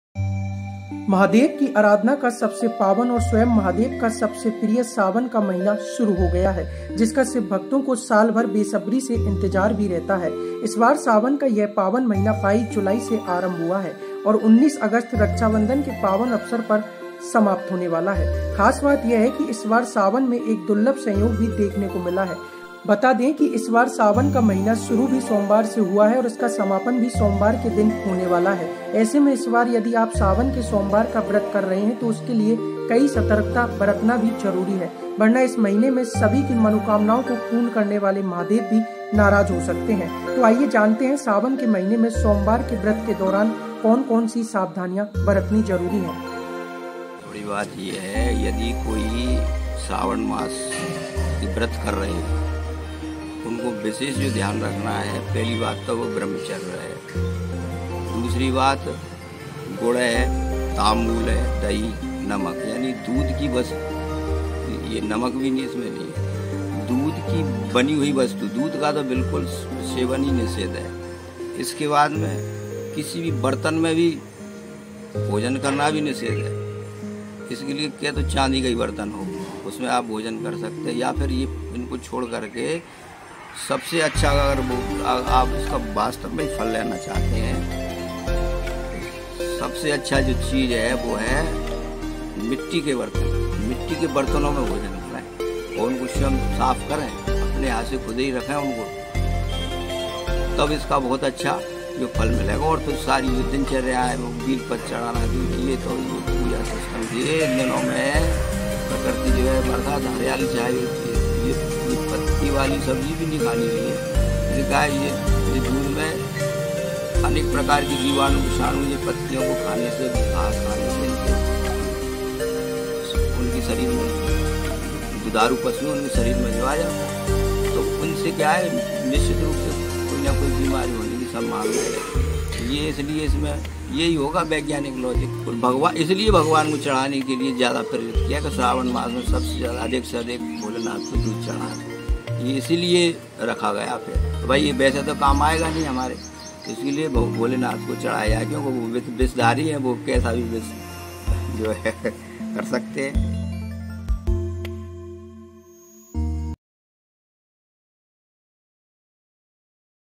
Sawan 2024: जानें सावन के Somvar Vrat के दौरान इन चीजों का सेवन क्यों है वर्जित?
Sawan 2024: जानें सावन के सोमवार व्रत के दौरान इन चीजों का सेवन क्यों है वर्जित? | Sawan Somvar Special

During the month of Sawan, special precautions need to be taken while worshiping Lord Shiva and observing Monday fasts. It is important to follow celibacy and avoid white items, especially milk, yogurt, jaggery, and salt. Eating in regular utensils is prohibited, and only silver or clay utensils should be used. Green vegetables should also be avoided due to the presence of harmful bacteria and viruses during the rainy season. Following these precautions ensures the blessings of Lord Shiva.
सावन महीने में भगवान शिव की आराधना और सोमवार के व्रत के दौरान कुछ खास सावधानियां बरतनी होती हैं। इस दौरान ब्रह्मचर्य का पालन करना और सफेद वस्तुओं, विशेषकर दूध, दही, गुड़ और नमक से दूरी बनाए रखना महत्वपूर्ण है। बर्तनों में भोजन निषेध है, और केवल चांदी या मिट्टी के बर्तनों का उपयोग करना शुभ माना जाता है। हरी सब्जियों का सेवन भी निषेध है क्योंकि बरसात के कारण उनमें जहरीले जीवाणु और विषाणु हो सकते हैं। इन सावधानियों का पालन करने से भगवान शिव की कृपा प्राप्त होती है।